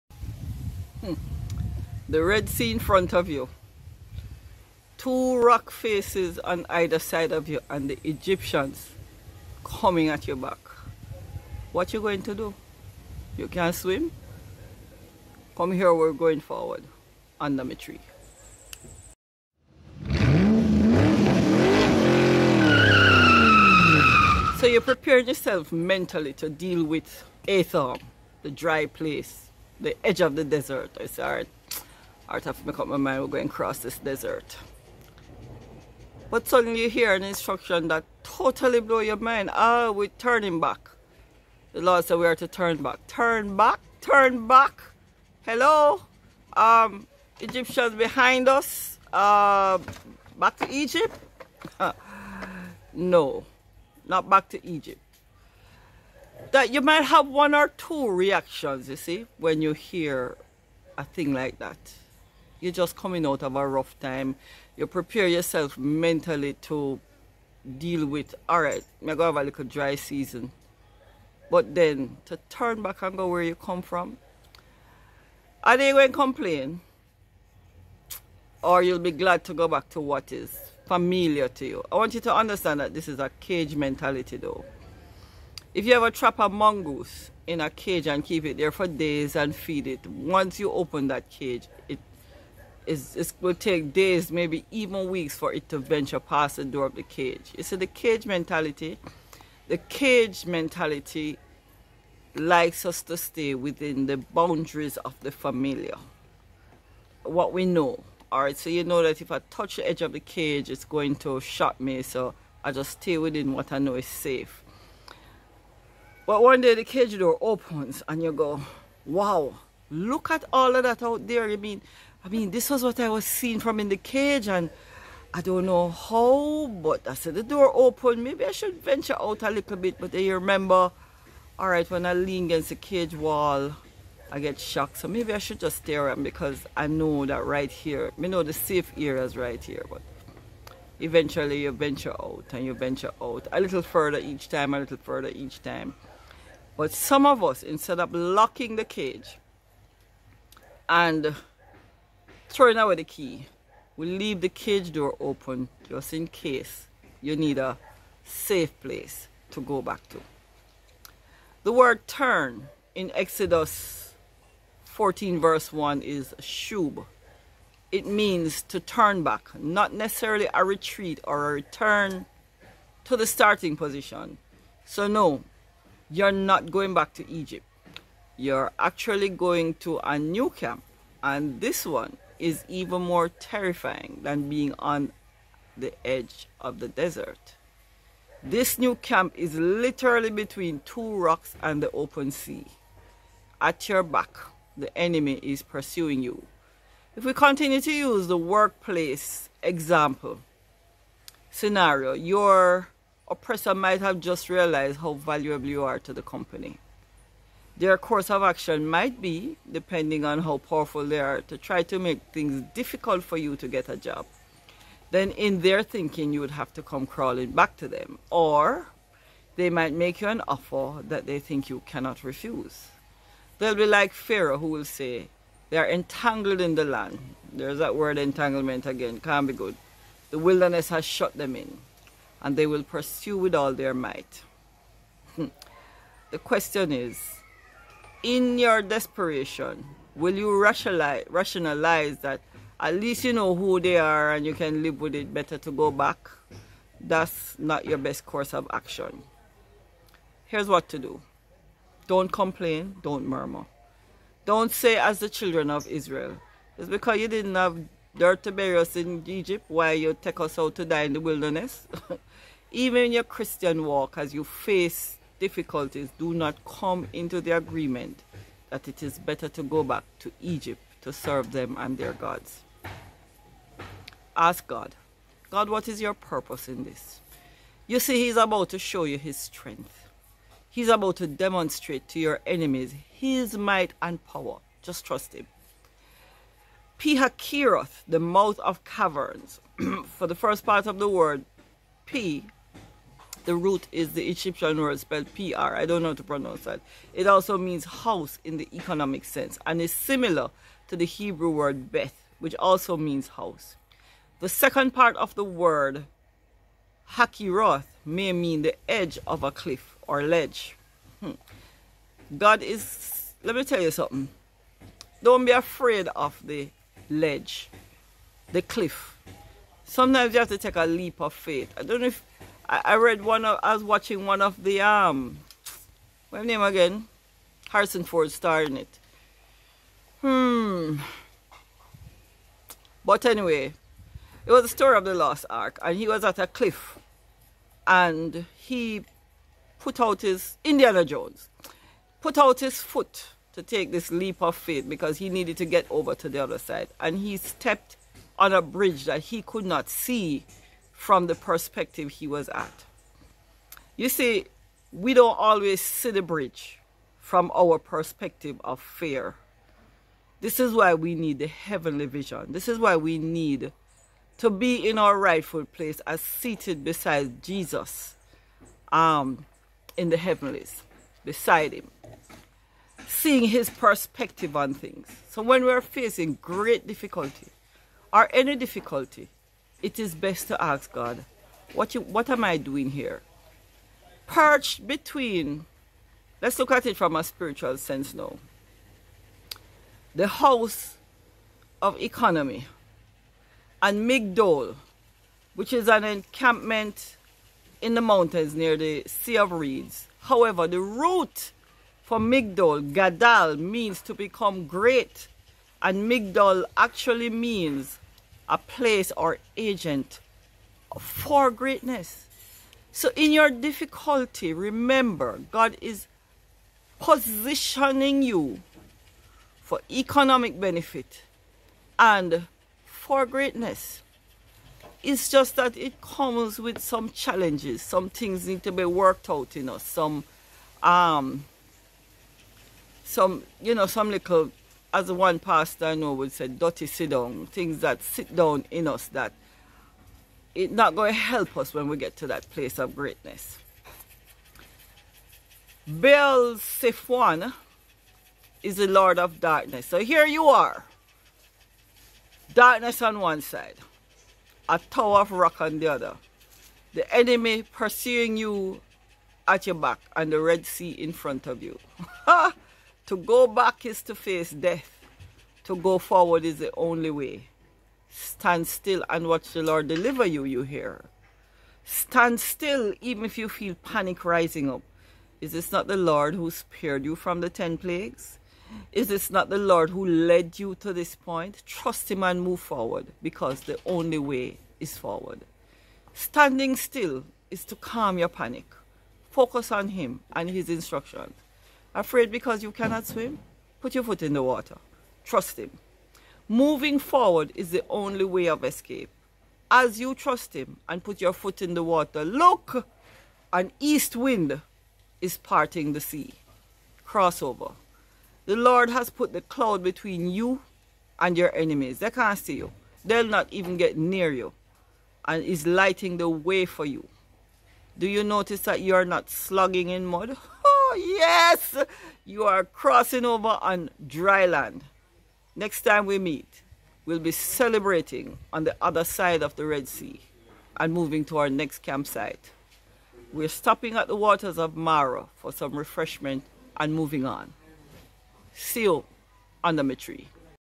Episode #6 - UNDA MI TREE. Journey Part 3 - Hemmed In
A rock wall on either side, The Red Sea before you and the enemy at your back - Can you swim?  Come Unda mi Tree to learn to navigate this journey